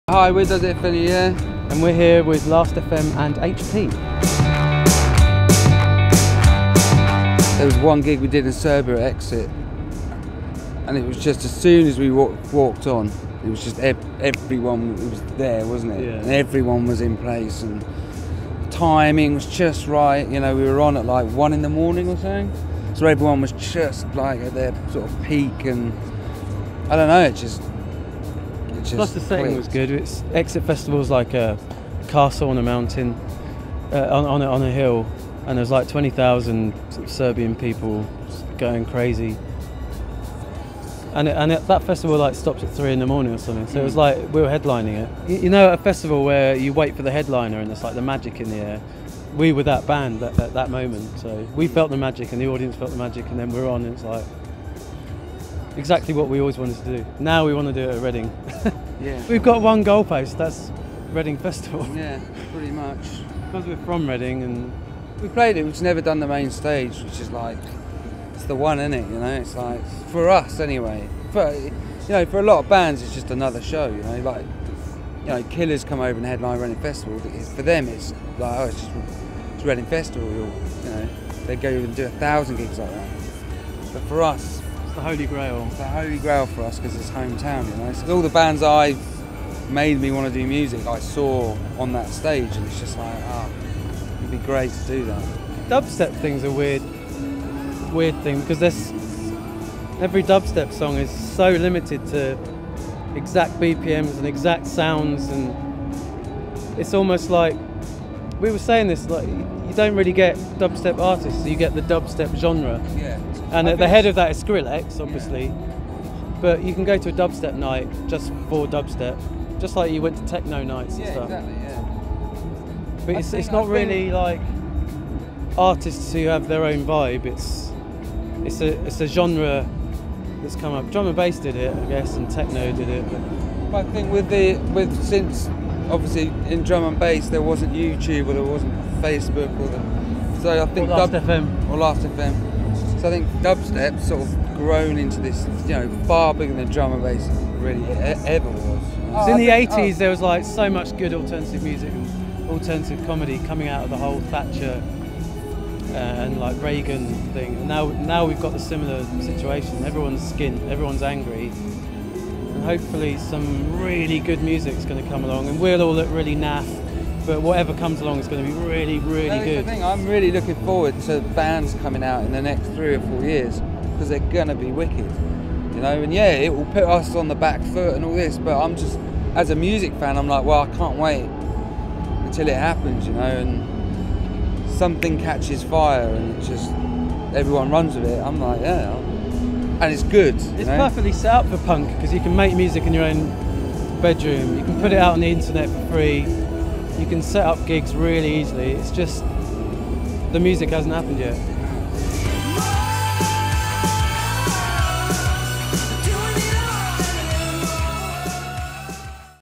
Does It Offend You, Yeah? @ Freeze Festival 2011 for Last.fm
The Last.fm team interview Does It Offend You, Yeah? at Freeze Festival 2011